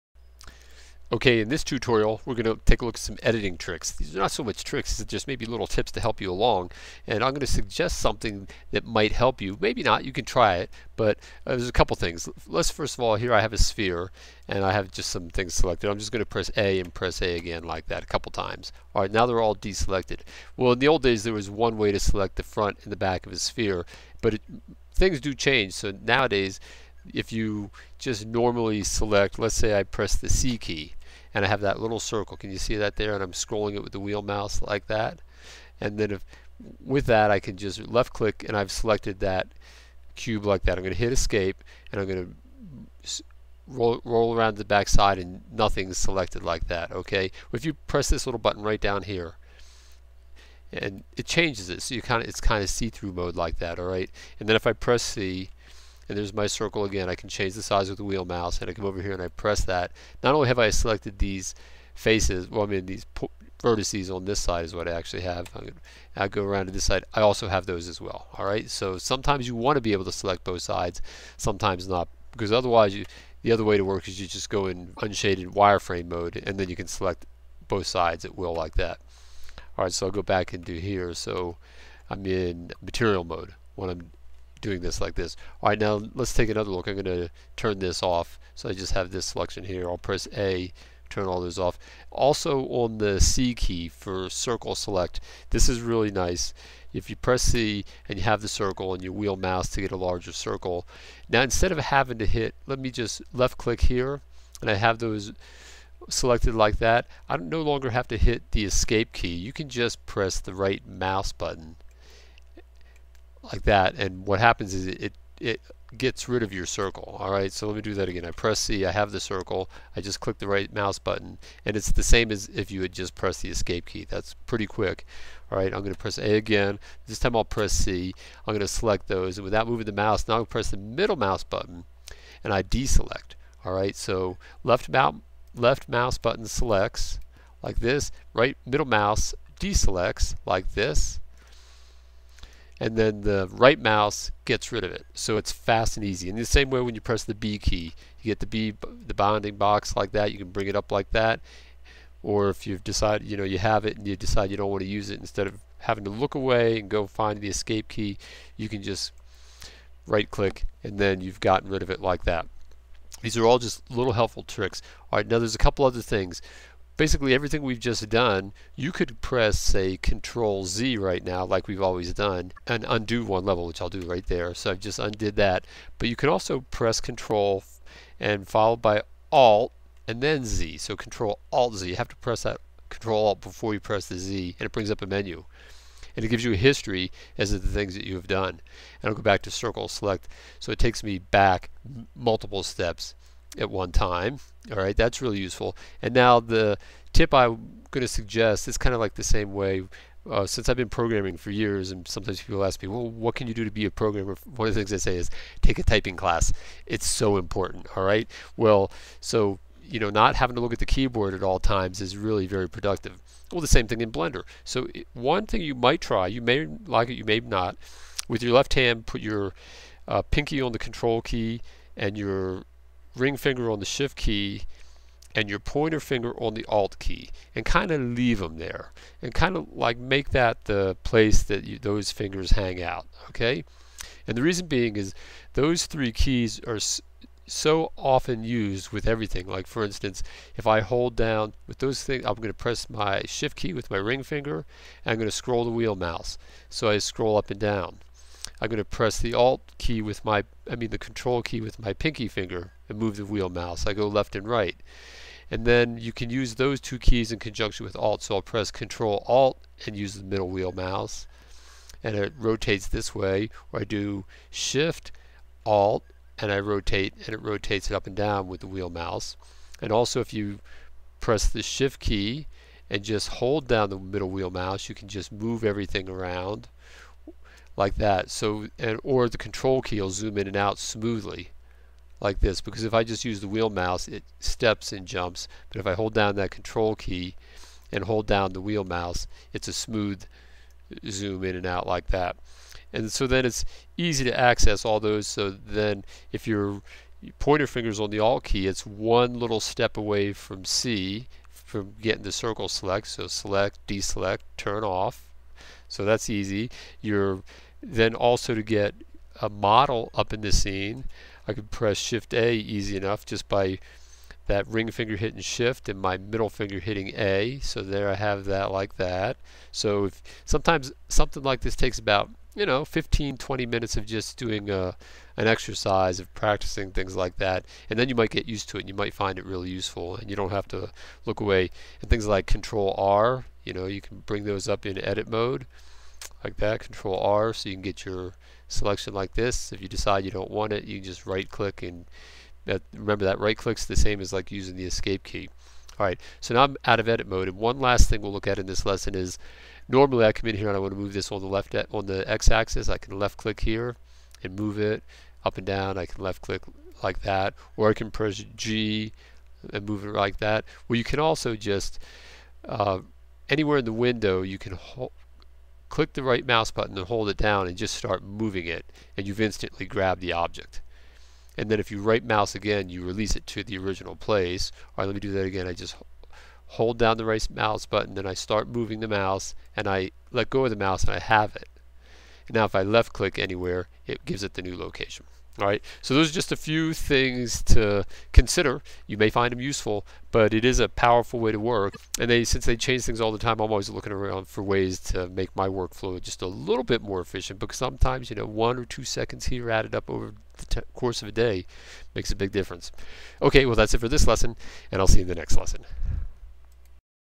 Blender Tutorial - Power User Editing Techniques
In this tutorial for new Blender users, I show you some editing techniques that might help you speed up your workflow. Blender 2.65.